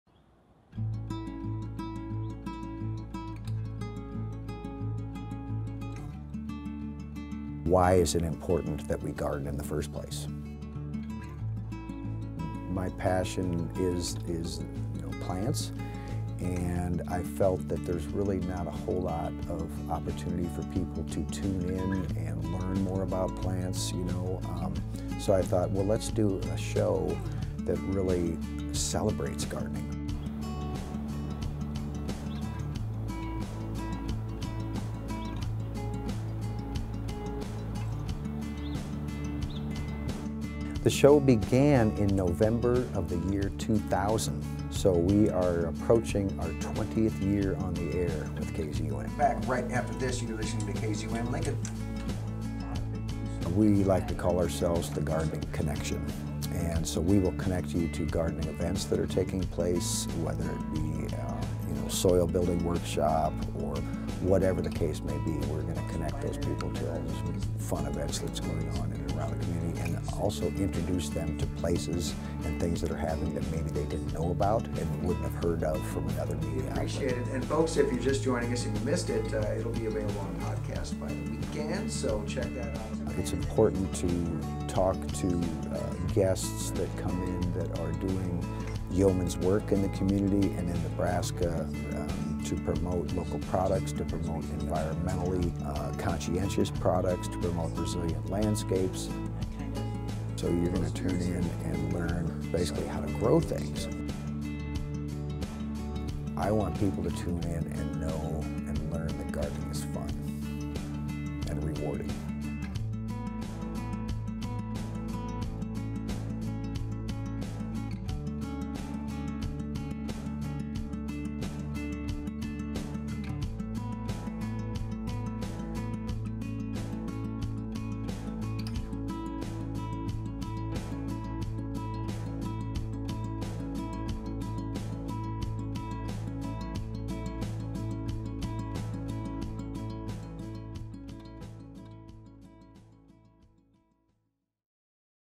The Gardening Connection: Radio host’s friendly demeanor, vast knowledge creates gardening community
Radio host Bob Henrickson talks about his show “How's it Growin’?” The radio show aired in November 2000 on the local radio station KZUM Lincoln. After starting as a talk show about seasonal plants and gardening, the show has now grown into a weekly program that consults local experts and botanists about the usage and benefits of certain plants and herbs and teaches the public how to grow and care for their own gardens. The radio show now hosts Garden Talk, Nature Walk events throughout the year, where listeners can visit local farms, plant nurseries and farmers markets to learn about what events are taking place during that time of year. Henrickson plans to continue to grow his Garden Talk, Nature Walk program and teach more people about the benefits of learning about sustainability and the importance of nature in the world today.